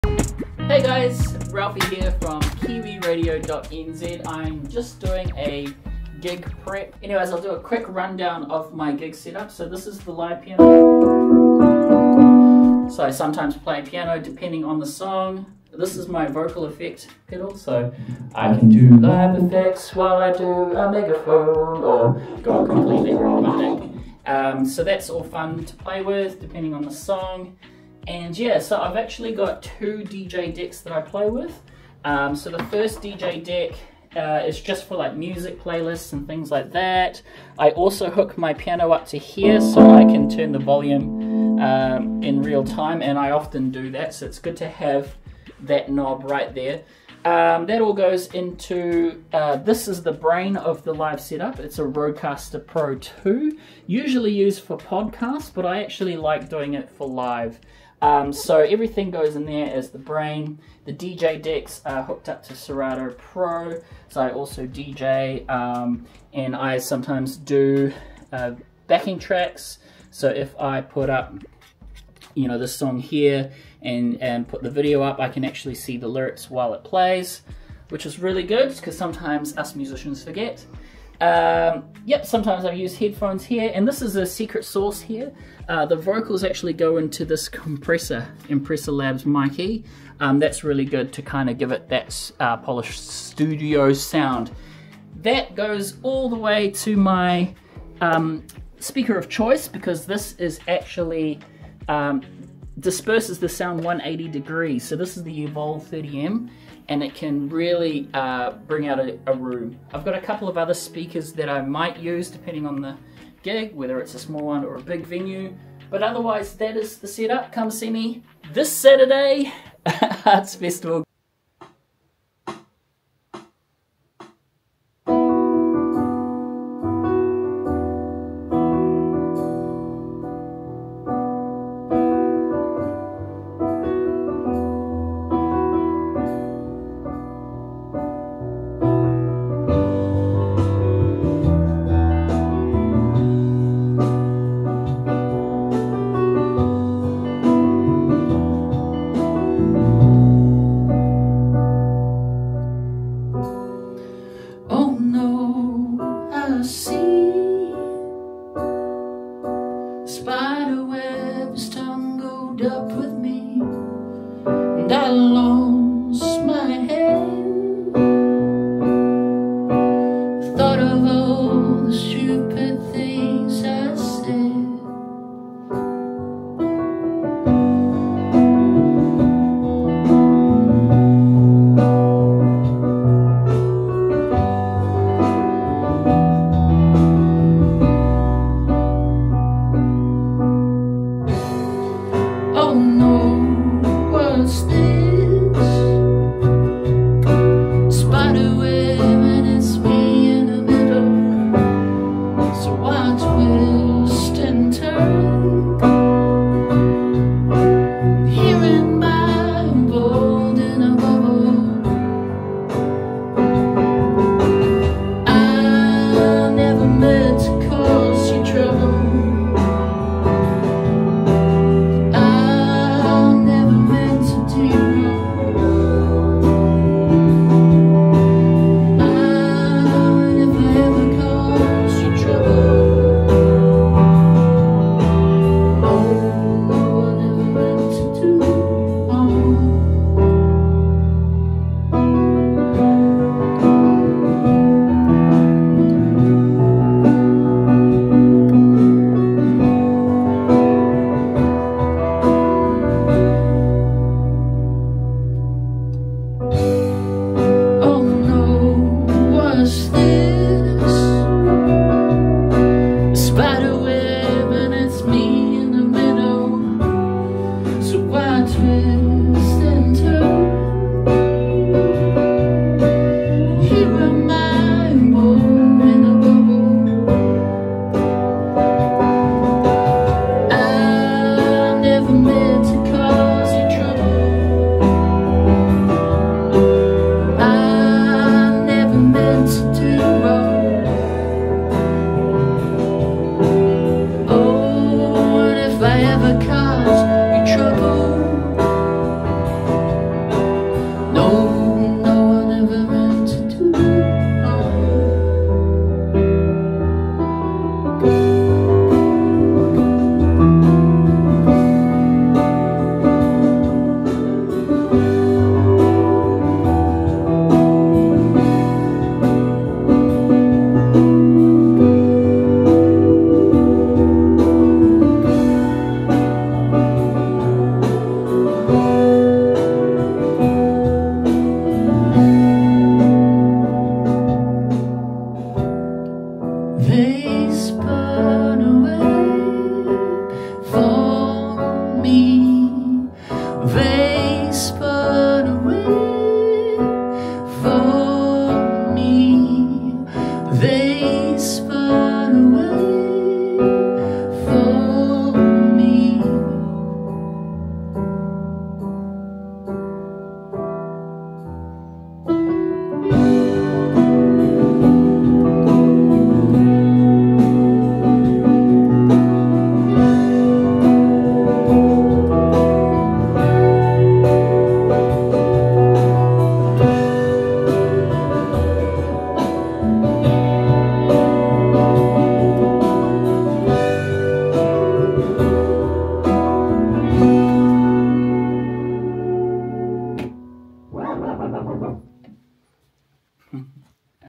Live Gear Rundown - Ralphe.co.nz - catch me live this Saturday Greenhithe Auckland
Here is a gear rundown of what I use live. Catch me live at Art Street Fair this Saturday 1 June 2024, 9am to 2pm.
Video Camera: M. Redmond